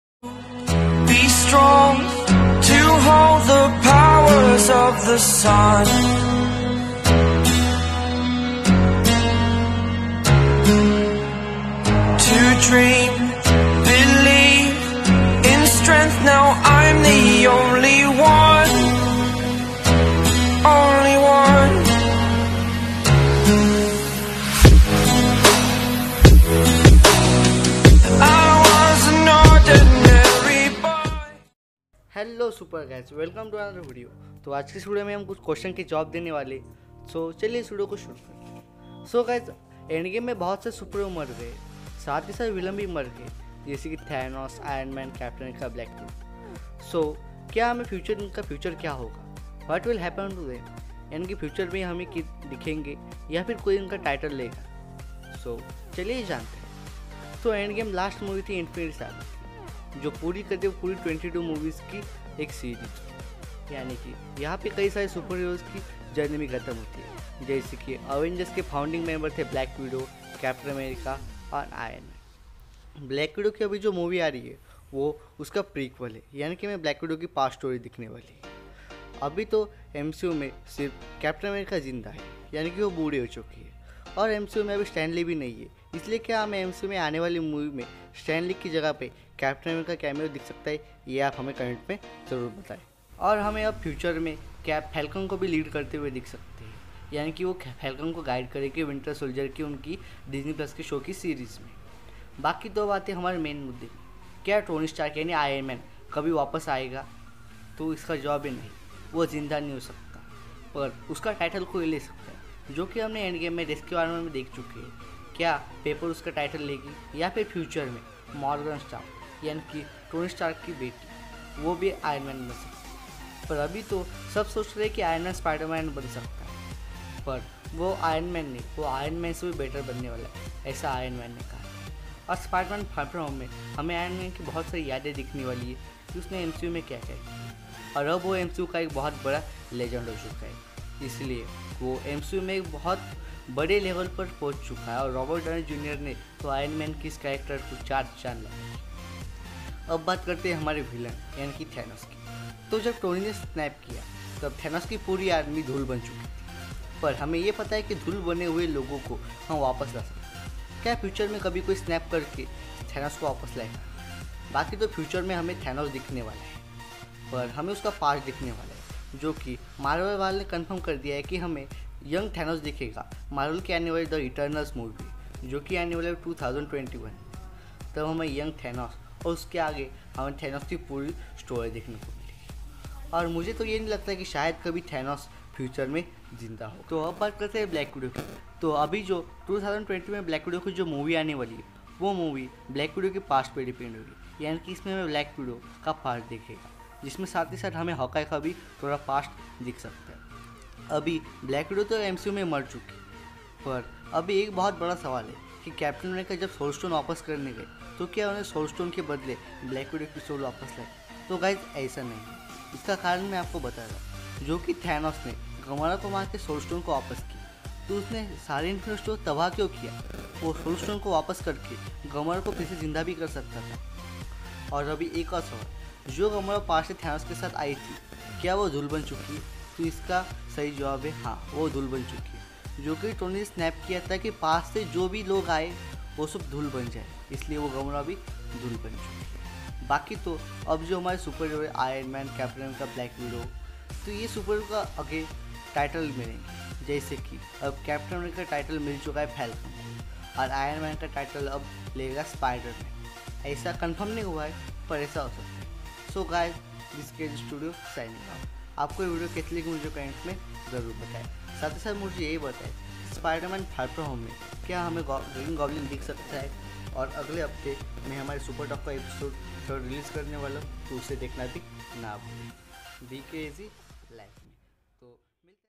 Marvel Phase 4 What will happen to Iron man, Captain America, Thanos and Black Widow
Hey Crazy guys tho ajki is video maine kuch Superheros kai future ka barame bataya hai to video Ko last tak dakhna aur Video Ko like karke subscribe karna na bhule

Video links

Be Crazy Like Me